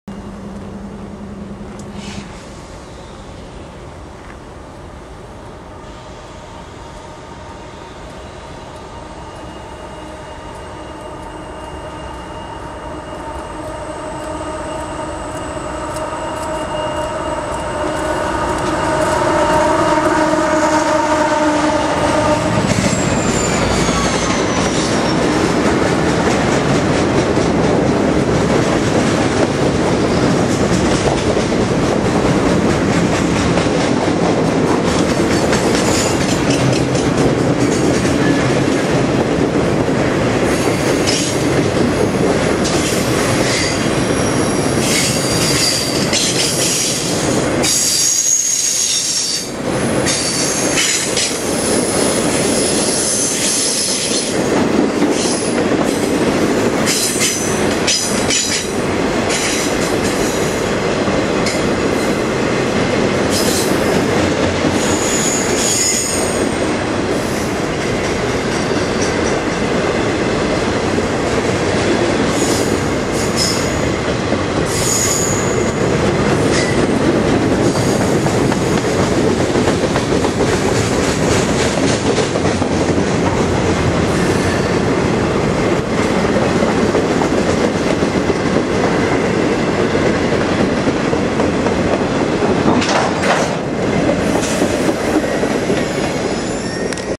CSX 315 Leads CSX W836 "DODX" at Coffey's Cliff
12/22/2008
10:50 A.M. ET
CSX W&A Sub
North Boyce

Chattanooga, TN

CSX W836 is the 2nd in line to come north on the 25 MPH slow order.